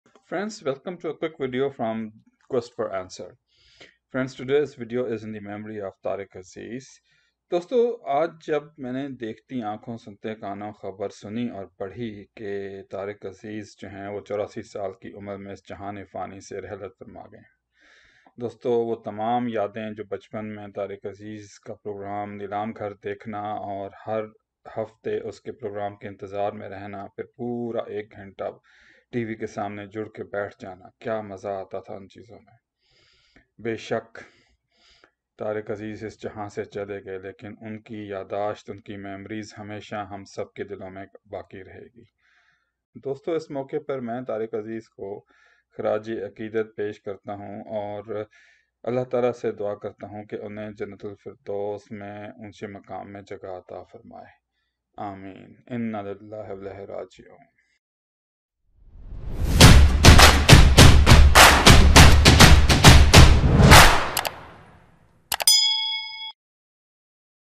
In the memory of Tariq Aziz & Nilaam Ghar | Quest for Answers
Welcome to Quest for Answers Channel (QFA). Where Answers Become Videos. I make interesting videos based on Life Experiences.

Friends this video is in memory of Tariq aziz who passed away today at the age of 84. Friends he was great actor and TV program Host from my childhood. His program Nilaam Ghar was a weekly program which will always be alive in the hearts of many people. We use to wait whole week for that program. May Allah forgive him and grant him high place in Jannah.
inna lillahi wa inallah-e-raji'oon.